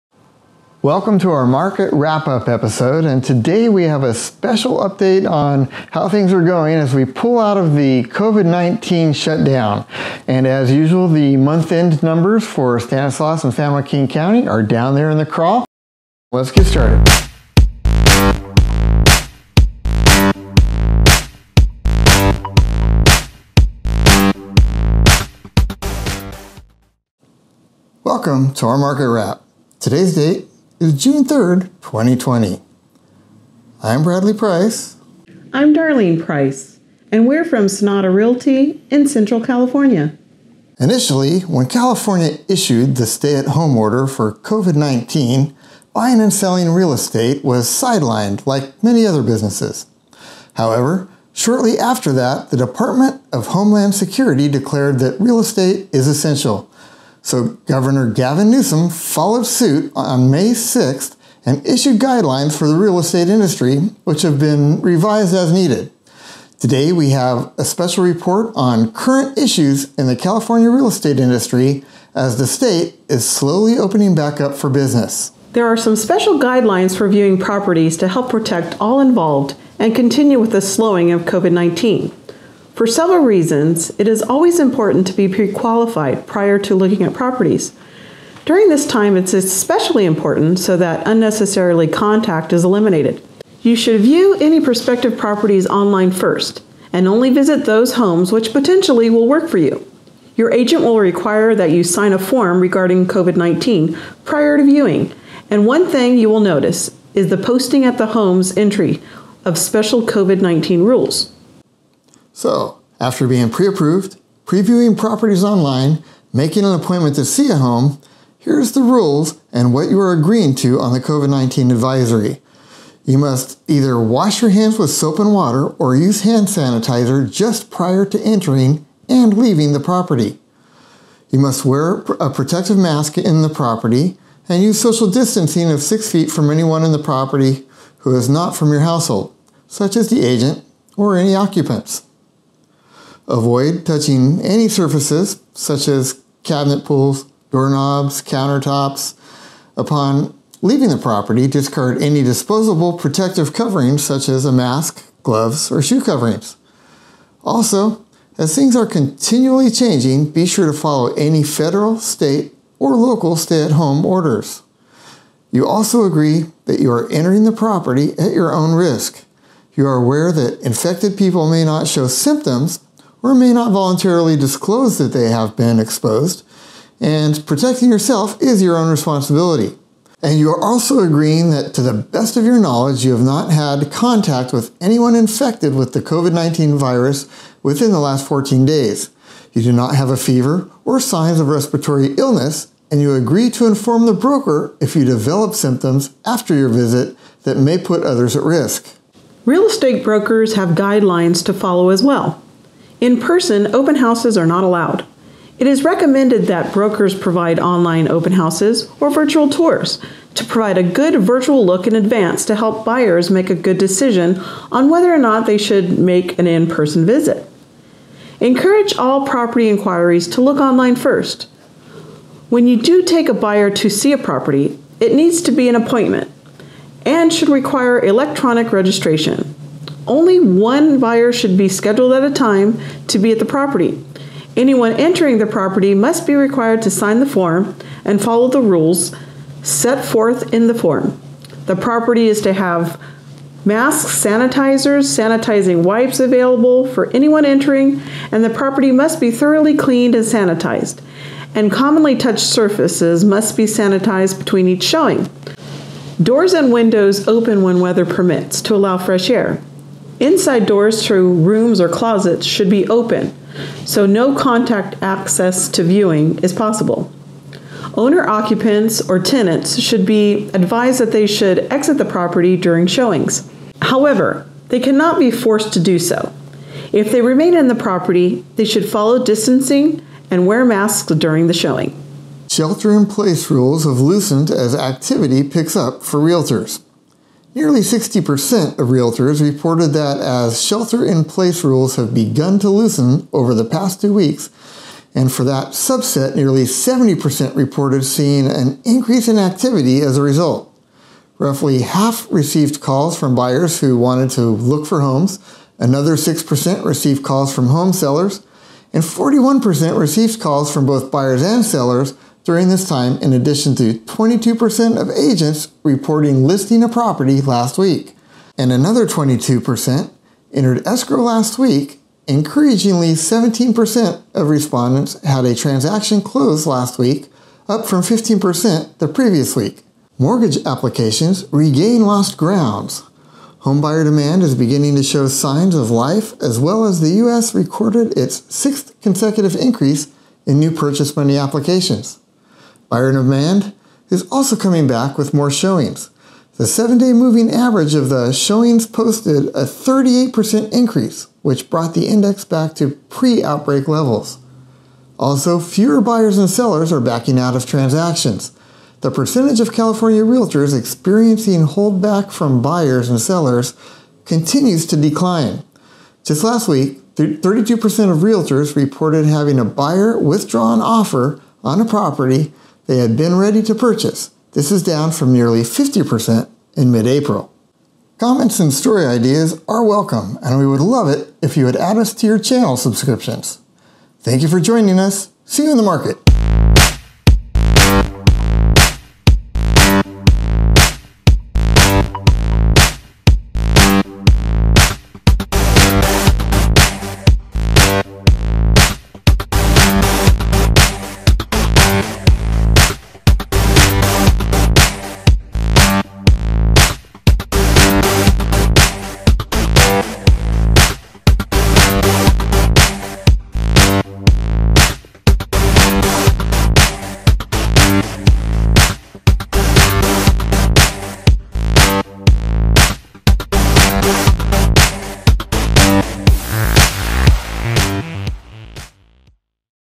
Real Estate News Market Wrap and in June 2020 Central California
News Style format with top real estate stories of June 2020, including the market sales statistics for May 2020 in Stanislaus County, and San Joaquin County, Including Tracy, Stockton, Manteca, Lodi, Escalon, Ripon, Modesto, Ceres, Turlock, Riverbank, Oakdale, Waterford. Today we discuss how the real estate market is operating as the state is gradually opening up sectors after a stay in place order for the covid-19 virus.  Keep informed in the market whether you want to buy a home, or put a home up for sale. Story Time  Market Wrap Up
May 2020 Numbers in the Crawl
 1:01 Ceres
1:13 Modesto
1:23 Turlock
1:42 Oakdale
1:54 Riverbank
 2:18 Waterford
2:30 Escalon
2:45 Lodi
2:58 Manteca
3:12 Ripon
3:25 Stockton
3:40 Tracy CA